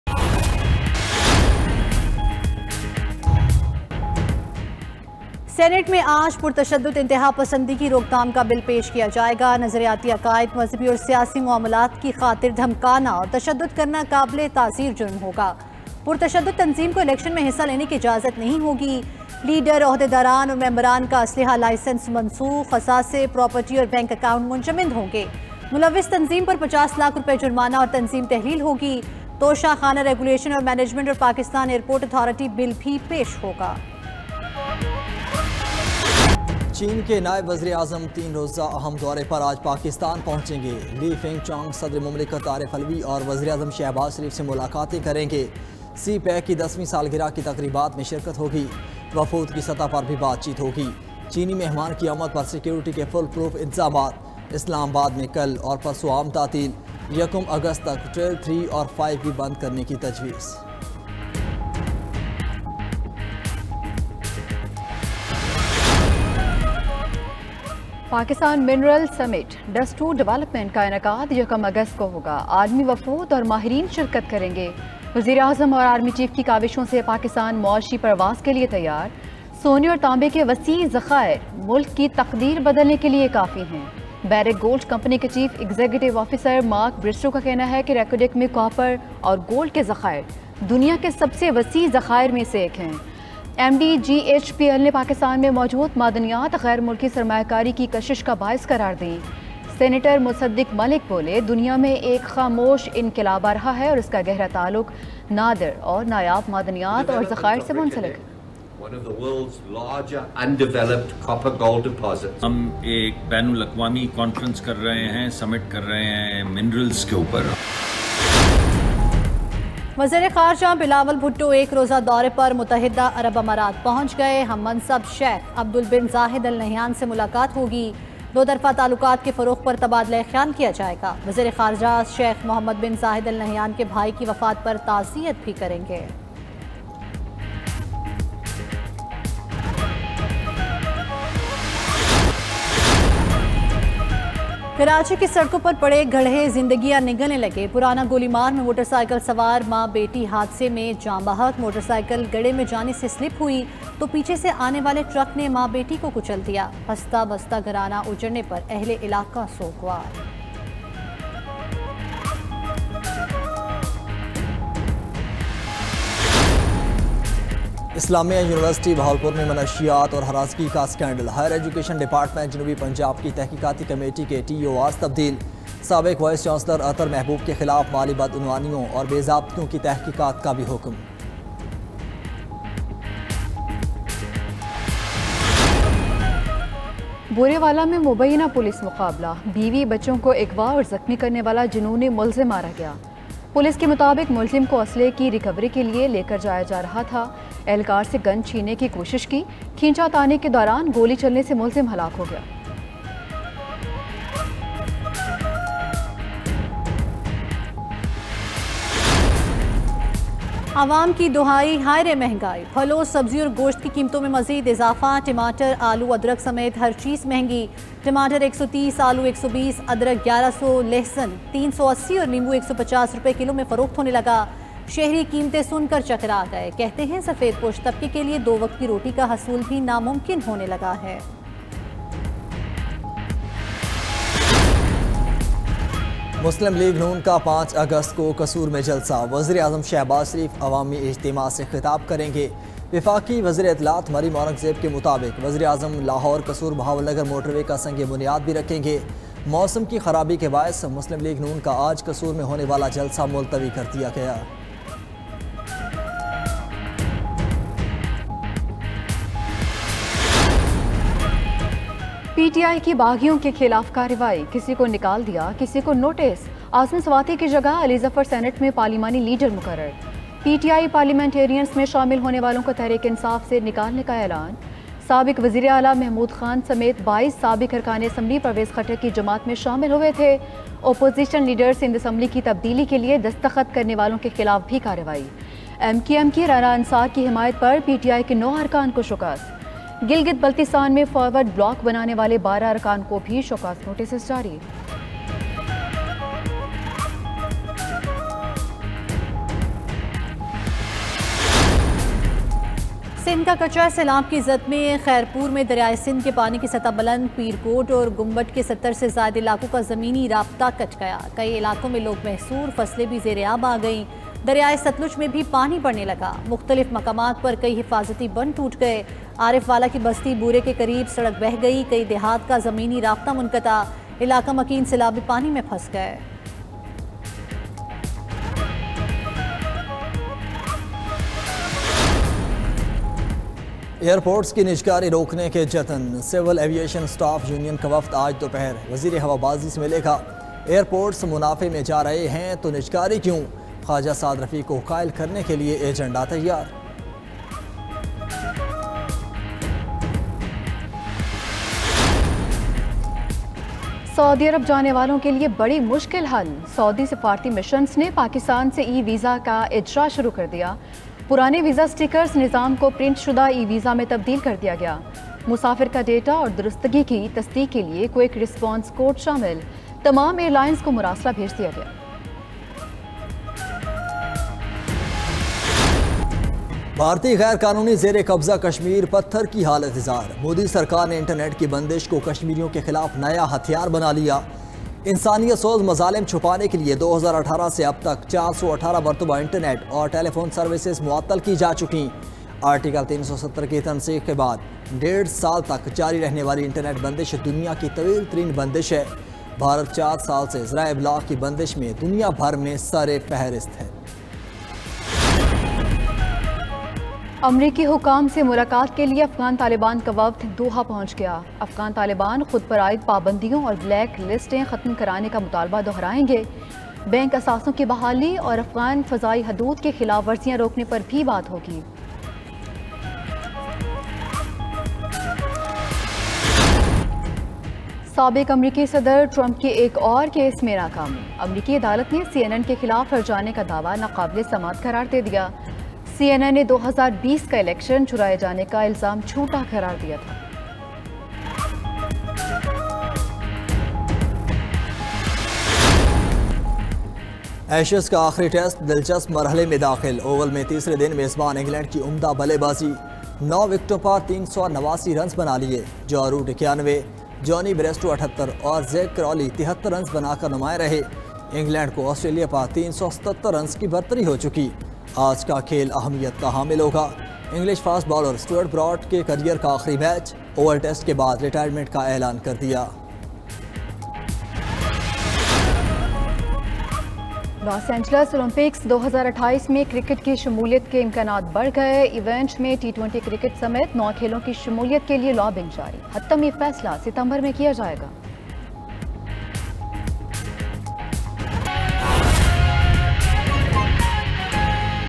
92 News Headlines 12 PM | Tehreek-e-Insaf out of the election? | 30 July 2023
92 News Headlines 12 PM | Tehreek-e-Insaf out of the election?  | 30 July 2023